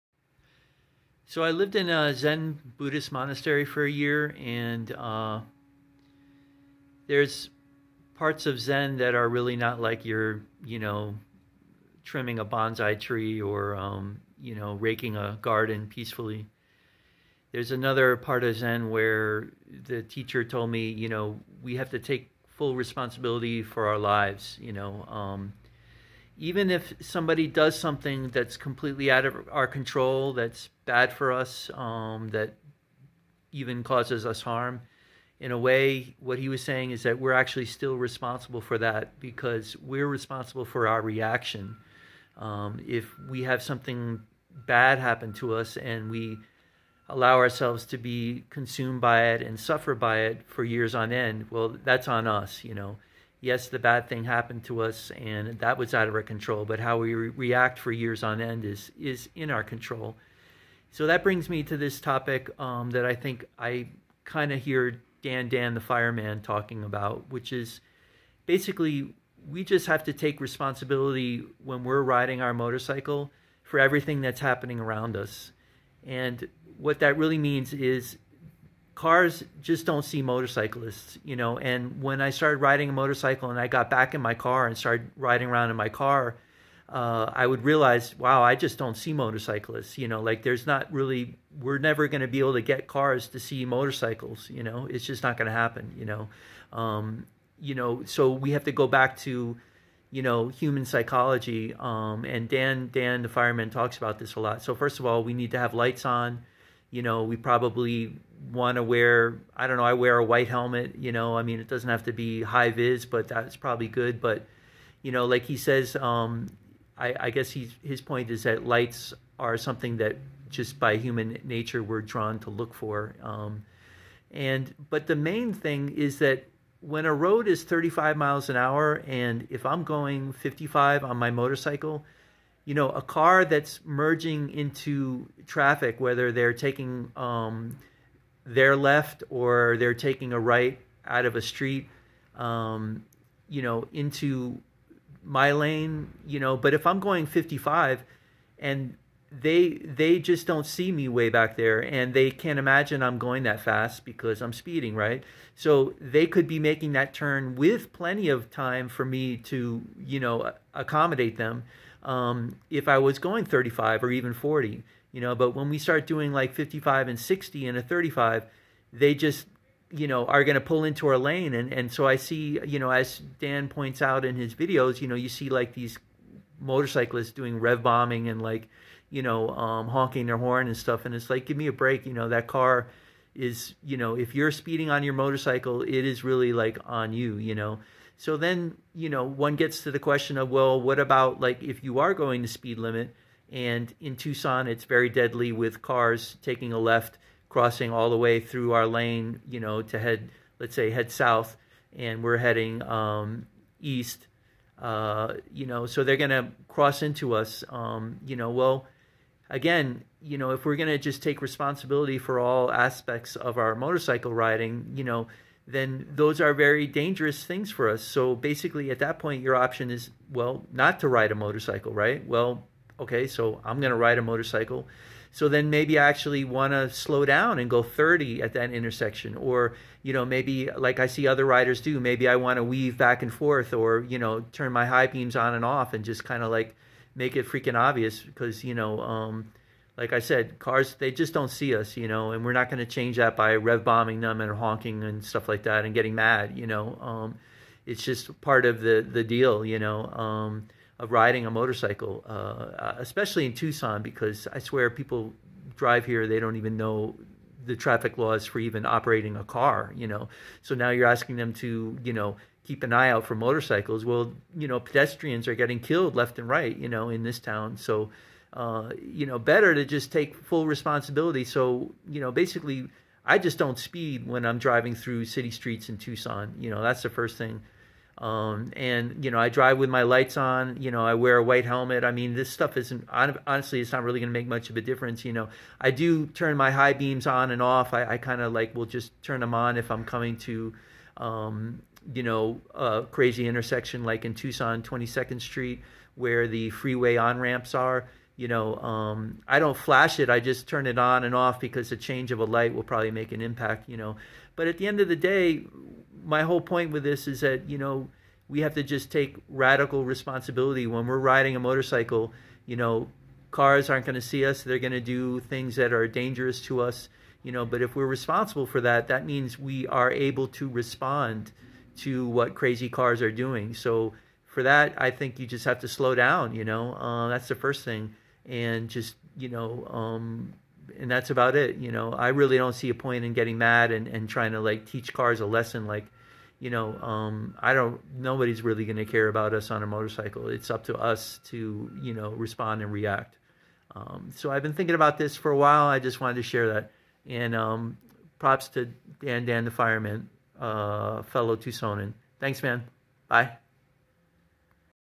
Zen Motorcycle like DanDanTheFireman - take radical responsibility for your riding
Radical Responsibility for the car driver.
I am responsible because I give myself the ability to respond to that driver, instead of wishing they would drive better, assuming they are going to see me. If I am going 50 in a 30 mph area, a car driver, who probably wouldn’t see me under the best of circumstance, even if they are vaguely aware some sort of vehicle is coming, will be used to all these other vehicles going 30 mph. If you are going 50mph, their timing that they know will work crossing the road as people are usually going 30mph, will be total off. First of all, remember they probably won’t notice you. But for sure, they won’t notice that you are going almost twice the speed limit. So this is our responsibility as a motorcyclist. I can’t make this drive pay attention, but I can at least put the odds in my favor.

I know I need to put these odds in my favor by going the speed limit, wearing a helmet, and understanding that if I am riding during these dangerous times of the day, maybe I take even more cautions.

Recently, a motorcyclist was killed in Tucson AZ, driving on a street I often travel on by car and by motorcycle. According to the police, the speed of the motorcyclist was a factor in this crash. This is an interesting street because on the one hand it seems like a pretty straightforward road to drive on. The car was heading from the north to the south and the motorcycle was driving from the West to the East. The car pulled into the motorcyclist lane, the motorcycle hit the car, and they died.
My philosophy is that even if the driver of the car, how they drive causes me an accident, it is not my fault, but it is my responsibility. Yes, the way the car driver chooses to drive their car is my responsibility. What I mean is that, even if they drive incorrectly, and I have an accident as a result, the accident is not my fault but it is my responsibility. The reason for this is because usually the motorcyclist while die in an accident with a car, so what point is there in getting mad at the way a car is driving? I’m the one that will die, not them. That’s like getting mad at a baby that poops in their diapers, they are a baby, that’s what they do. In a similar way, a person in a car just SIMPLY IS NOT PAYING VERY CLOSE ATTENTION, and all the yelling, complaining, and hope that drivers will look for a motorcycle is in vain. So, what would you do if two year old babies were driving cars?
Well, as someone on a motorcycle, we should understand some simple statistics. Three I keep in mind is that most motorcycle fatalities involve a motorcycle running into a car, they weren’t wearing a helmet, and they were riding during certain times of the day especially 3p to 6p. So my responsibility, my ability to respond, it to wear a helmet, be careful when I am riding, but, it seems simplest, don’t speed.